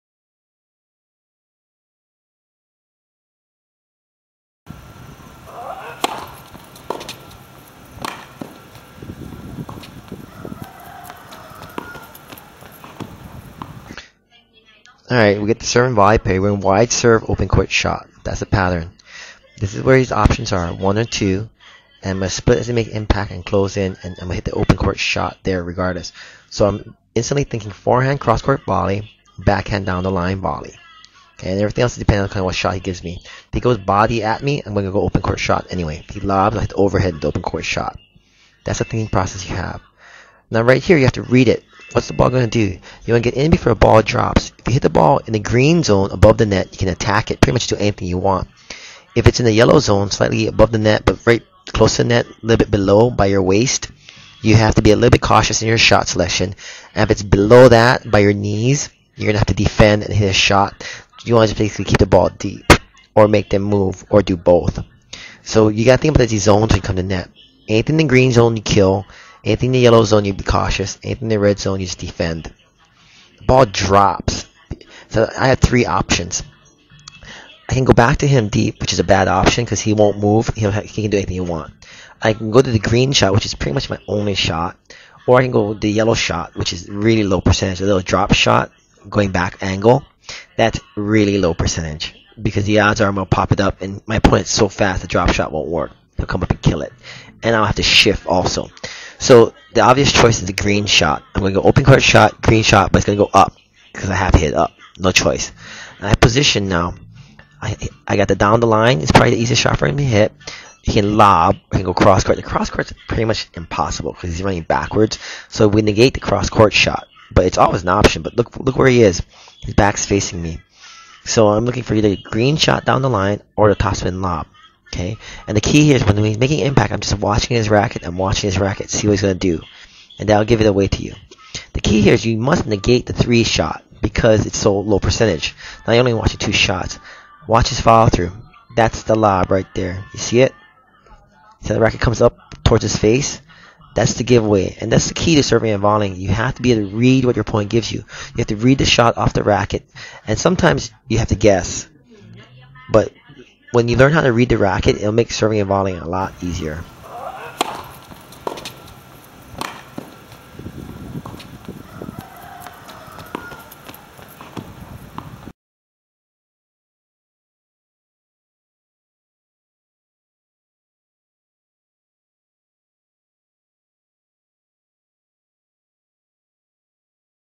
Tennis Lesson: The Second Shot in the Serve and Volley
shows options in the serve and volley